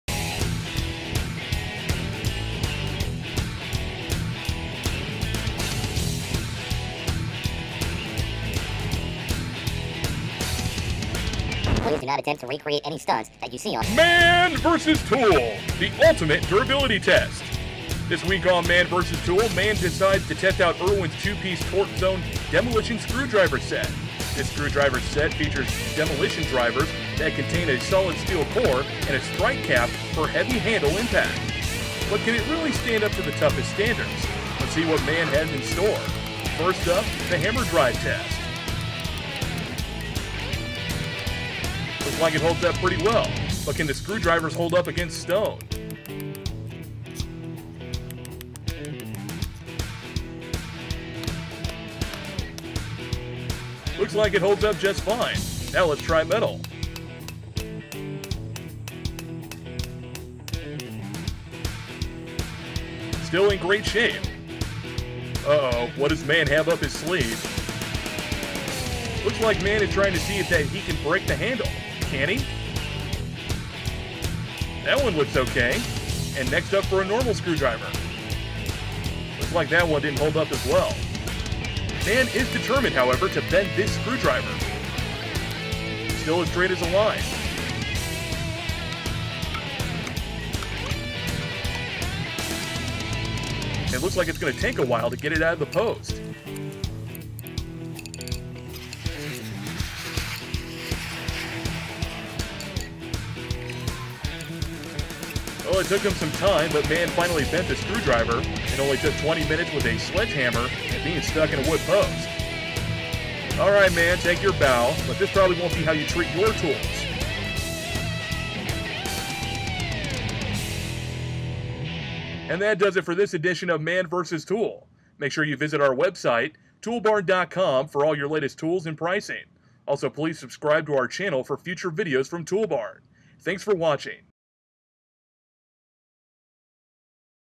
Man Vs Tool (Irwin Demolition Screwdrivers)
Irwin just came out with a great new line of screwdrivers.  Man takes on the demolition screwdriver and shows us just how tough it really is.